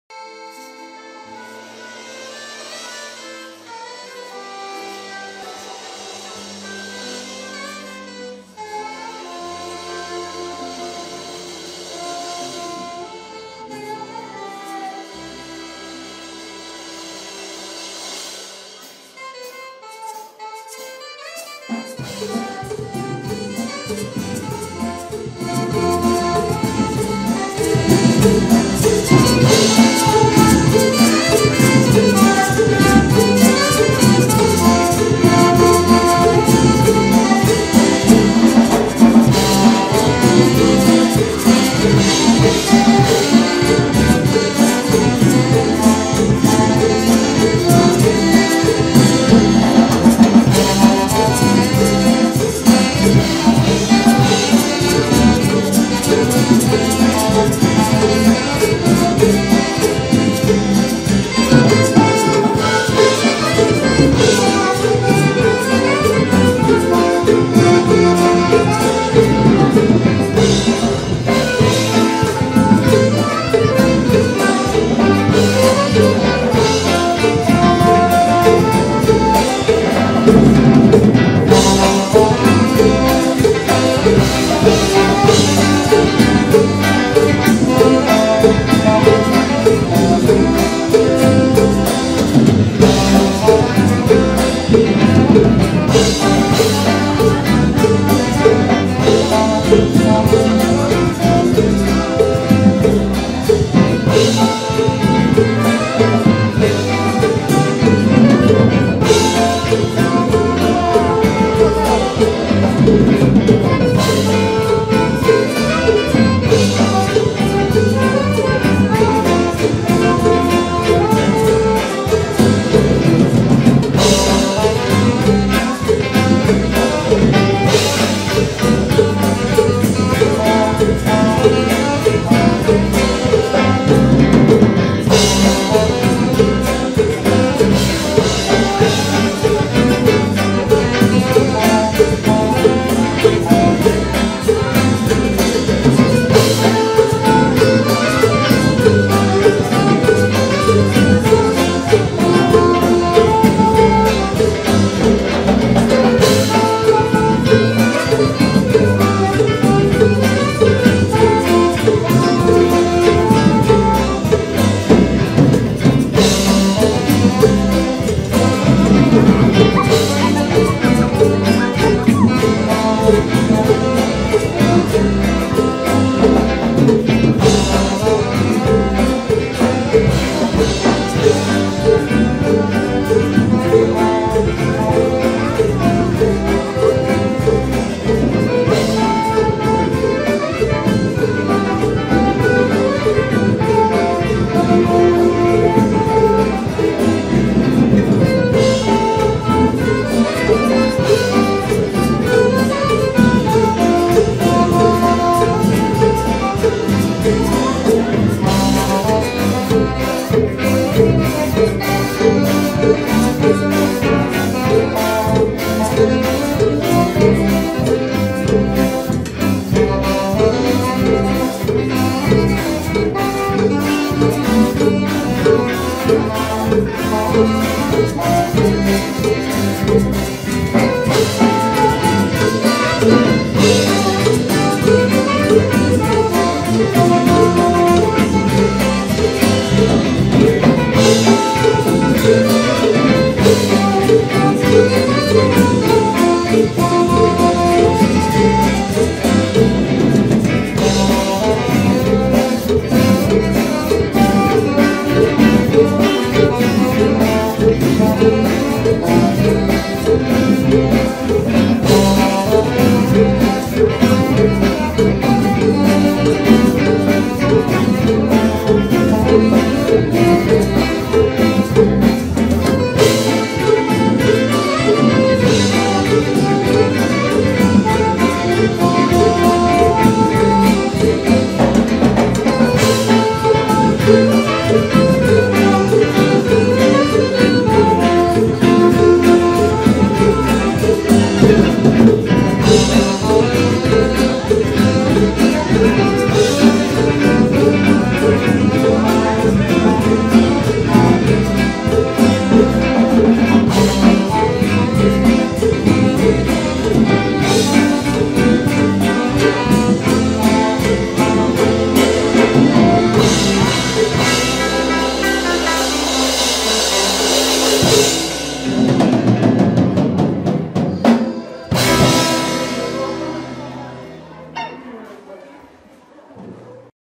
A Cumbia from T.O. Combo
T.O. Combo at the San Xavier, Arizona  2013 feast.